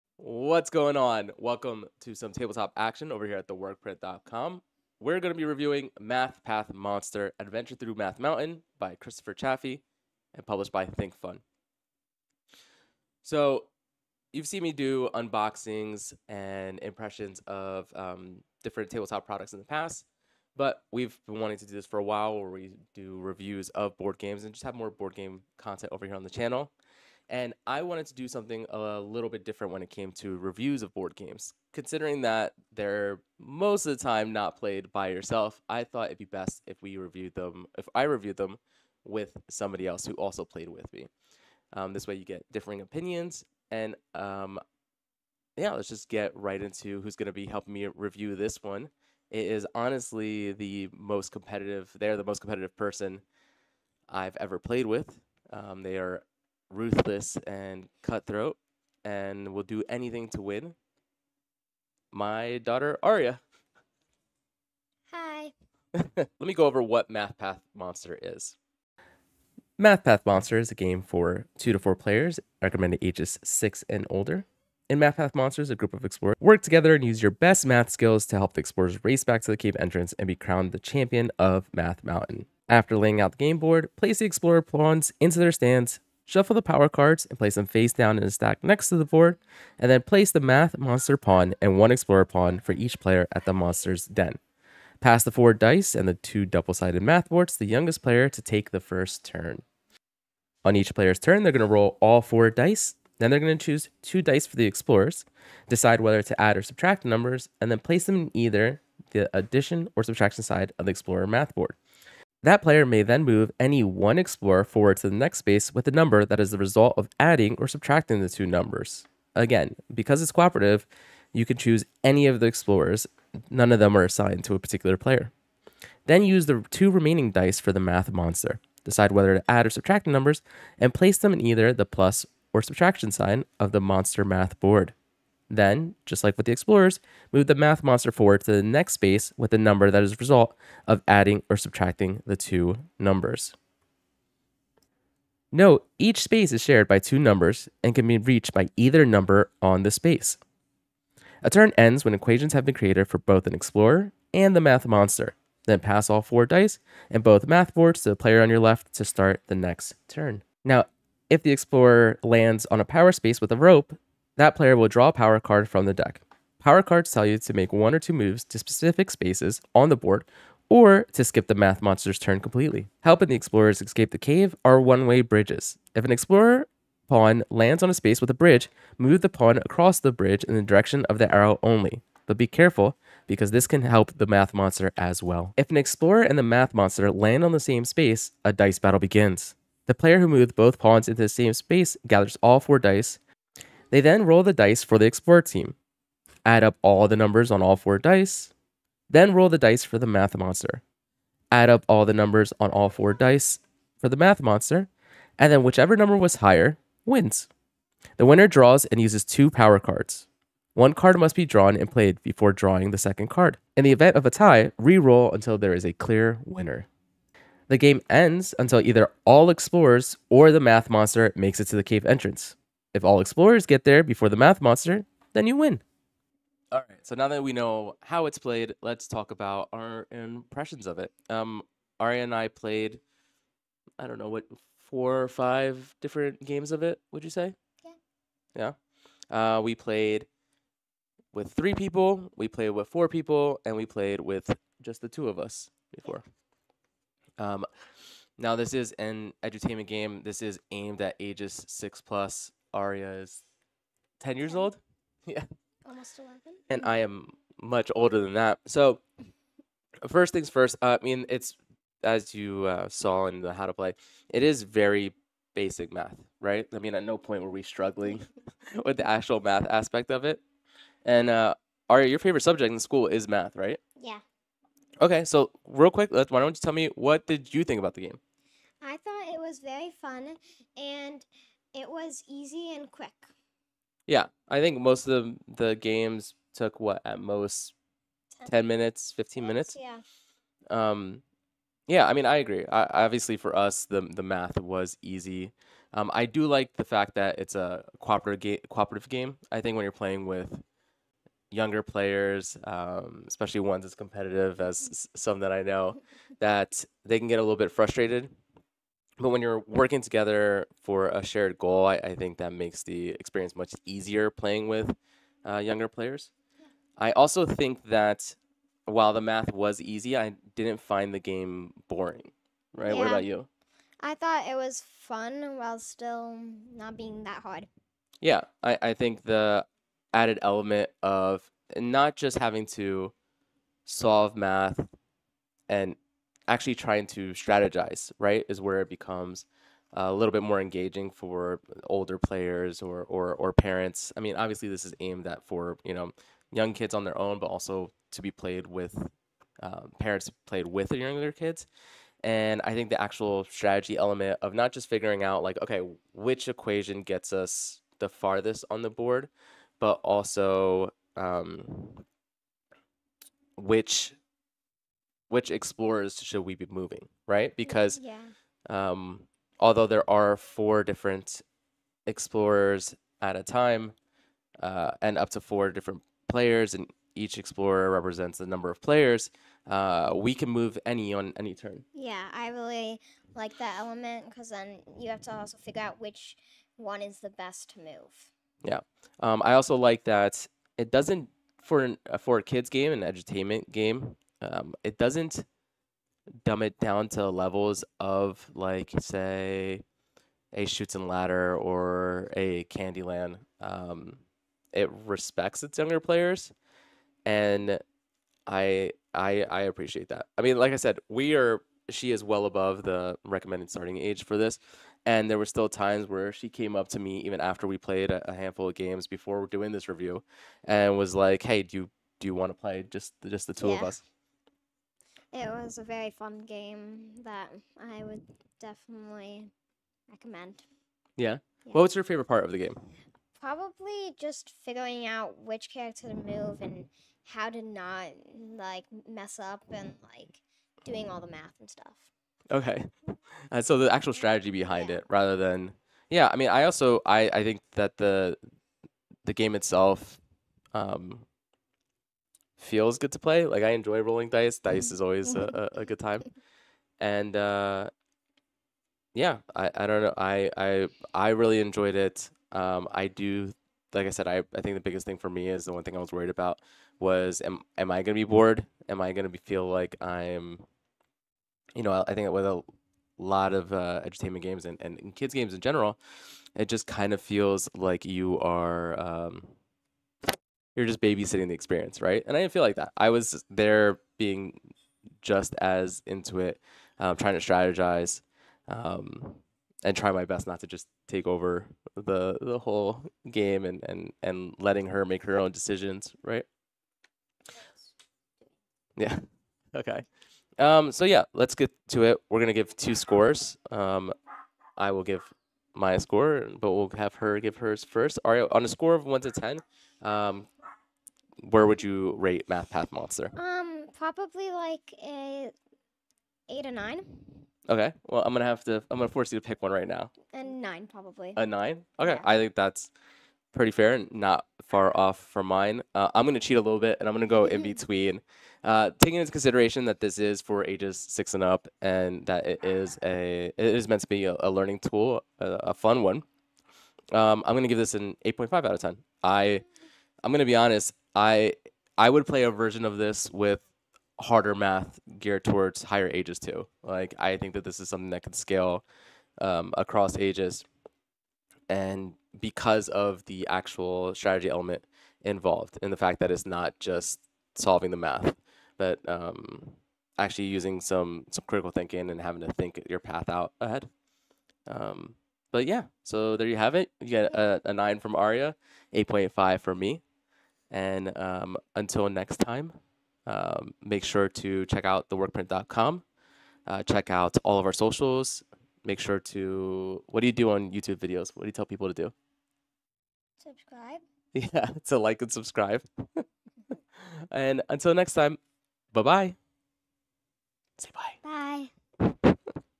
Math Path Monster- How to Play and Review!
Rob and a special guest review Math Path Monster by Thinkfun!